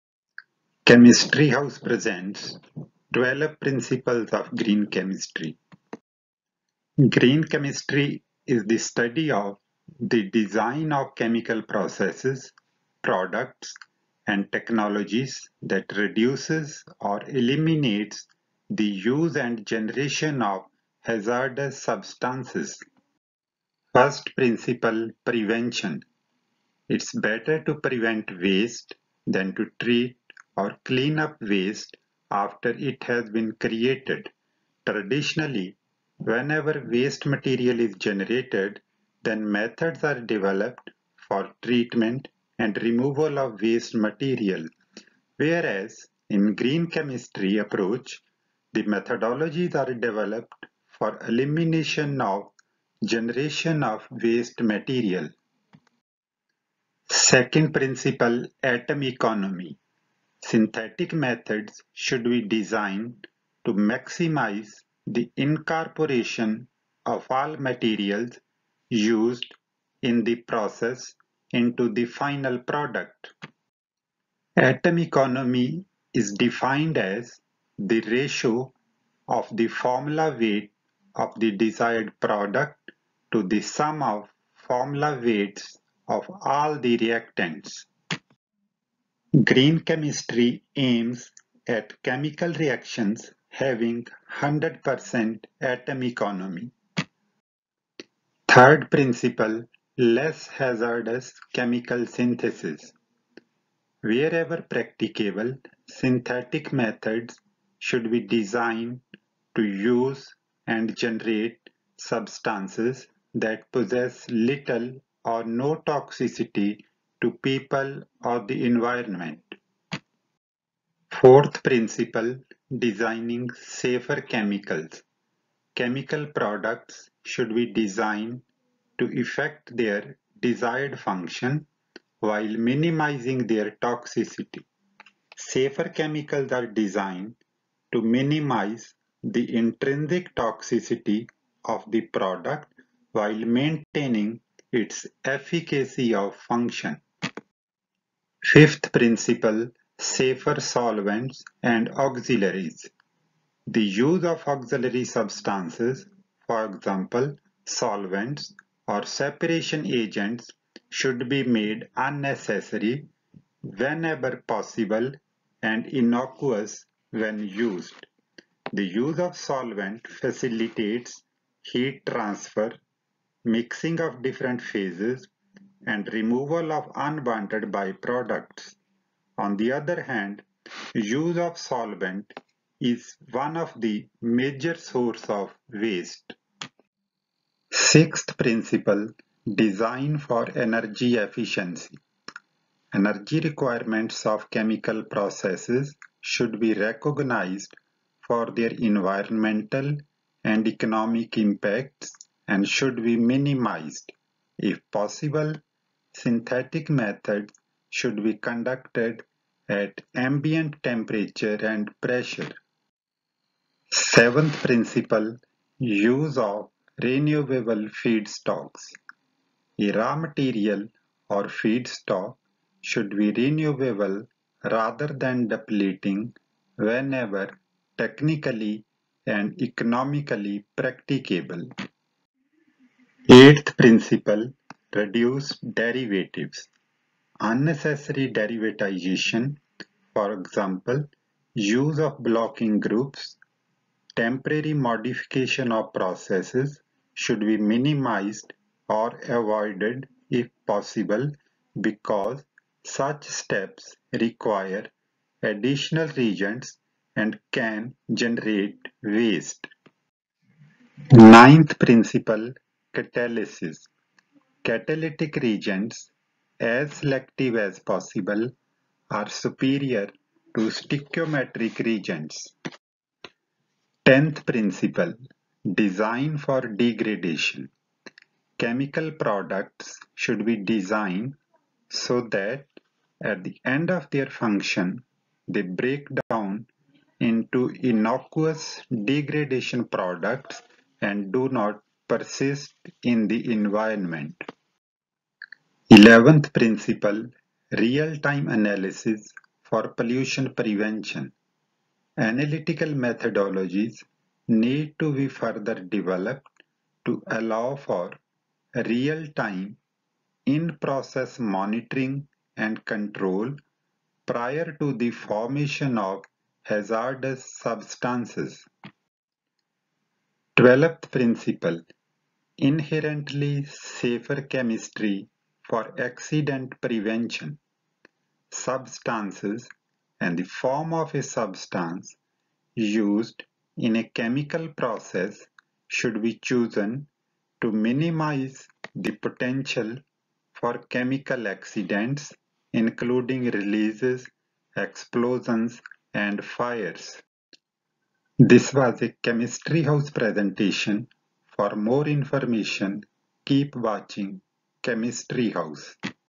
Principles of Green Chemistry
Green Chemistry is study of design of chemical processes, products and technologies that reduces or eliminates the use and generation of hazardous substances